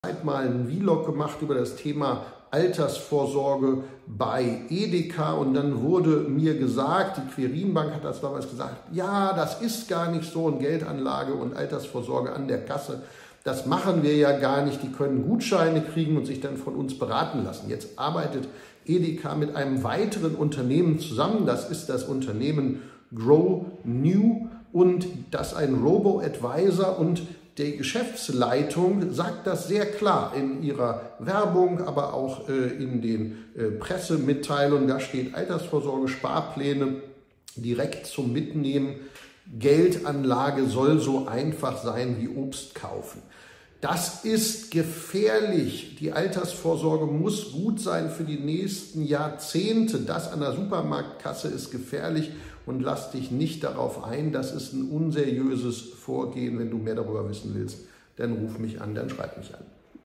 Altersvorsorge im Supermarkt zu verkaufen ist unseriös
Altersvorsorge im Supermarkt zu verkaufen ist unseriös 🔥 Was sind Deine Gedanken zu diesem Beitrag?
finanzielleallgemeinbildung 🔥🔥🔥🔥🔥🔥🔥🔥🔥 🚀 Du willst Partner in meinem internationalen Team werden👇 Vereinbare Dein kostenloses Strategiegespräch